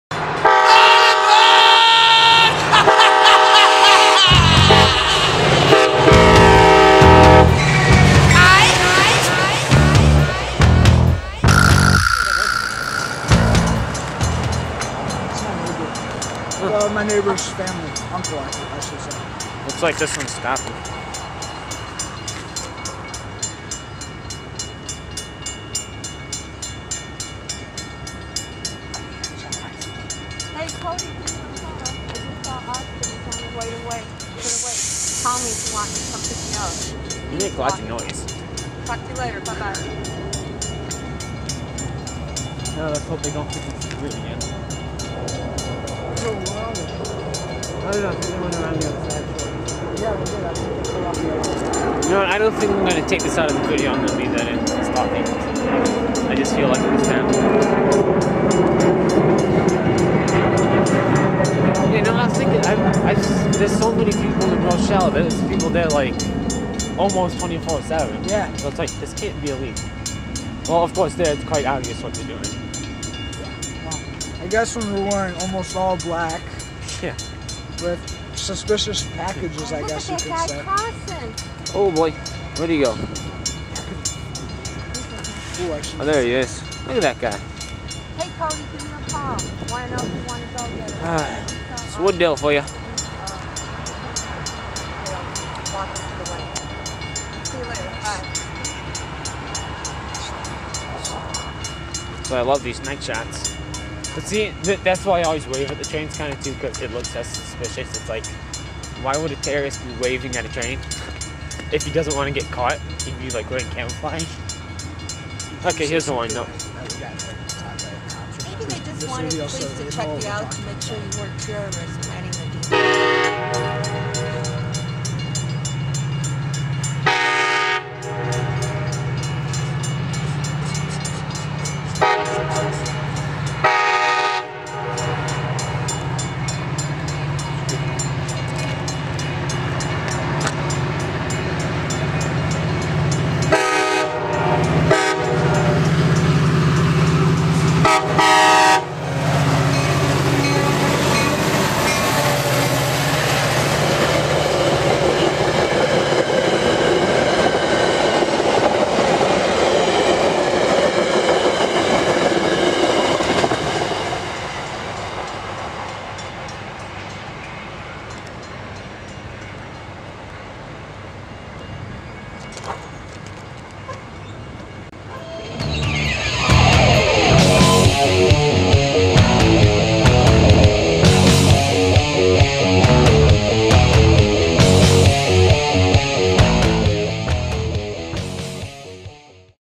Metra in Wood Dale with Tresspasser after cops lecture us about railfaning. (HD)
A trespasser goes across the tracks as me and Eric discuss the reasons that the cops had to talk to us, almost causing us to miss a train. READ WHAT HAPPENED HERE:
We got our first shot of the night in at Wood Dale Rd just after a squad car went by. They stopped in the Shorty's parking lot and walked over to us. They took down all of our information (age, names, date of birth, home address) then lectured us about how this could look like a terrorist act. This unfortunately adds another incident to my list of nearly illegal actions which has previously included an airsoft incident. I railfan Wood Dale all the time and get nothing but friendly waves and horn shows from the engineers so I don't see how this could be a problem.